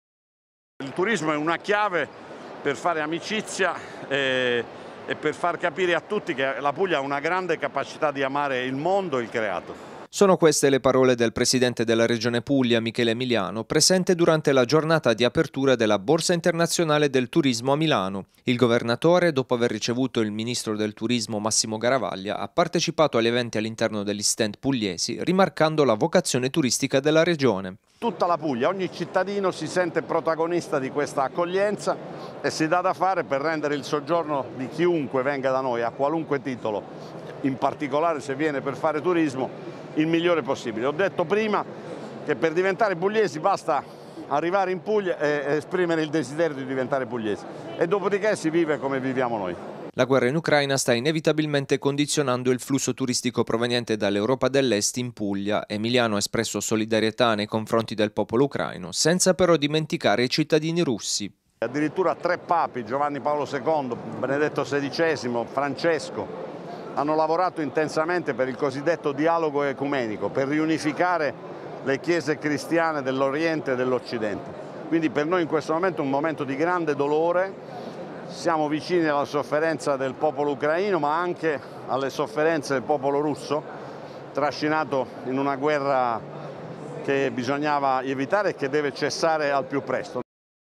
BIT 2022, Emiliano: "Boom del turismo merito dei pugliesi"
Il governatore pugliese ha incontrato gli operatori turistici durante la prima giornata della Borsa Internazionale del Turismo